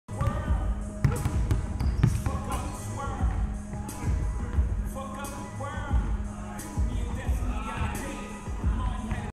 Tom "Bush" Wamukota (7'0 Center) with Stefan Moody
Tom "Bush" Wamukota (7'0 Center) at Back2Basic in New Orleans, LA with Stefan Moody.